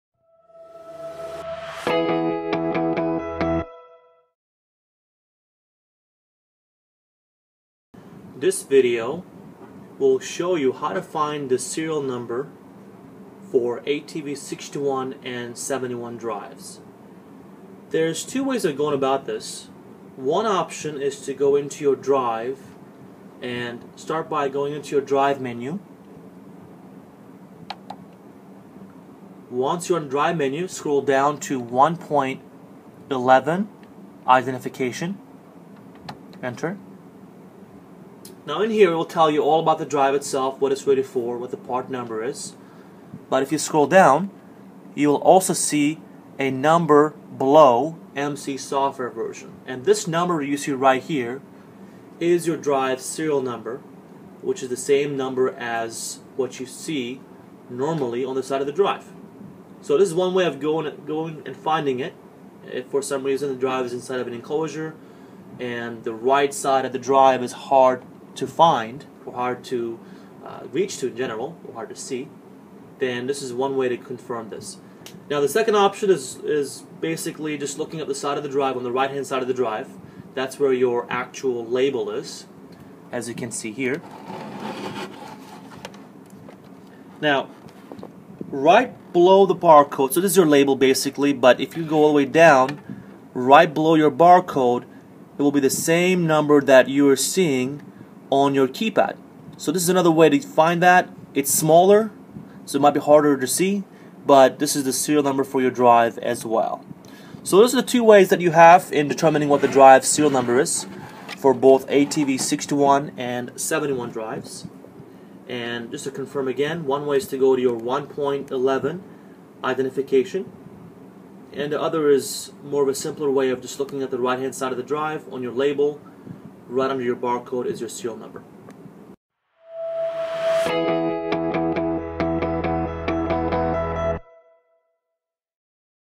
Locating Serial Number on Altivar 61 and 71 Series Drives | Schneider Electric Support
Tutorial for locating the serial number on the Altivar 61 & 71 (ATV61/ATV71) variable speed drives via the front display and product identification sticker on the enclosure.